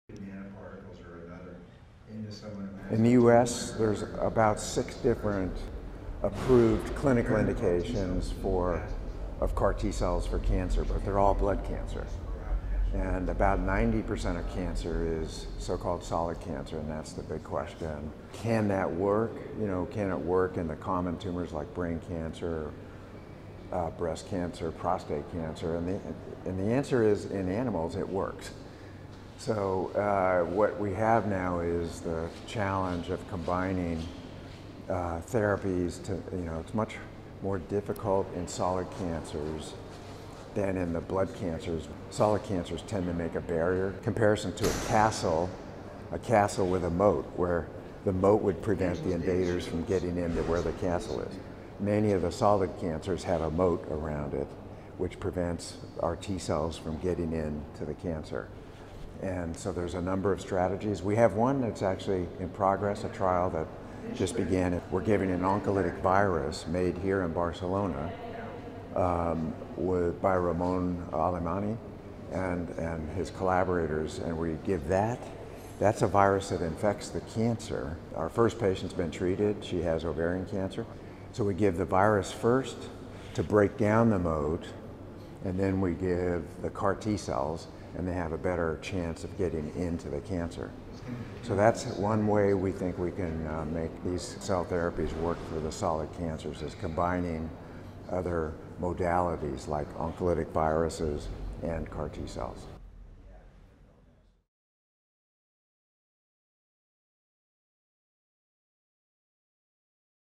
Interview to Carl June (2): What is the status of CAR-T cells studies in solid tumors?
On 9 September, Carl H. June, Richard W. Vague Professor at the University of Pennsylvania, joined the team of Manel Juan, head of the Hospital Clínic Immunology Department and leader of the IDIBAPS group Immunogenetics of the autoinflammatory response, to commemorate the 50th anniversary of the creation of ImmunoClínic. June is considered the father of immunotherapy to treat cancer because of his pioneering research in stimulating the immune system’s T cells to fight tumors.